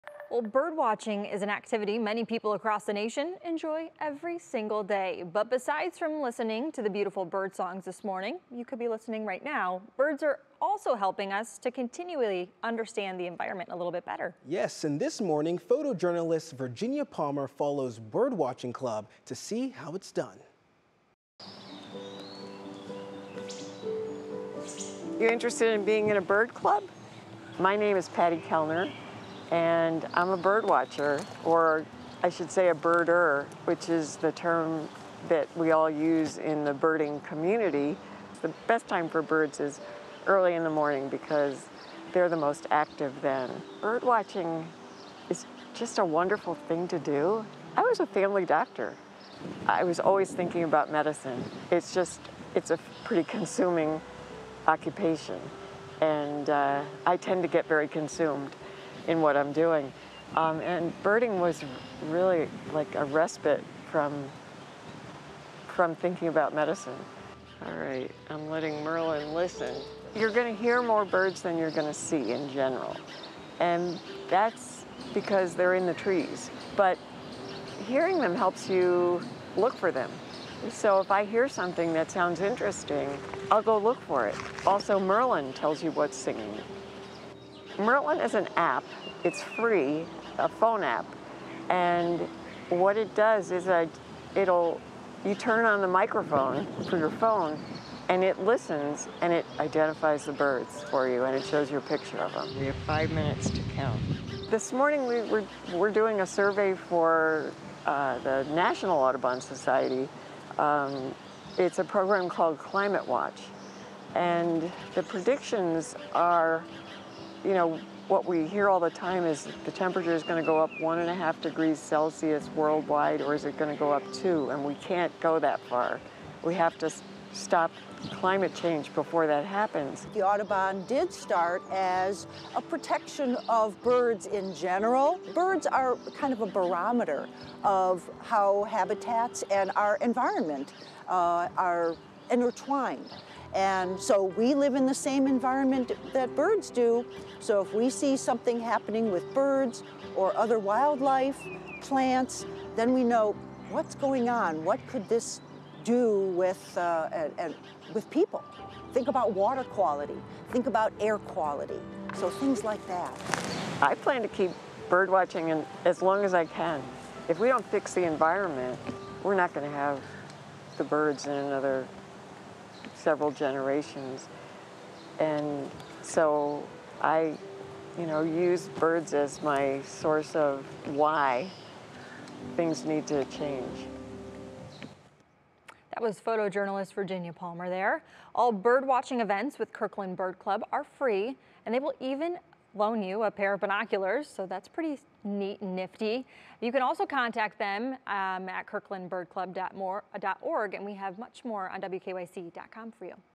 Northeast Ohio bird watching club helping individuals understand the environment
The Kirtland Bird Watching Club hosts free events for bird watching in Northeast Ohio.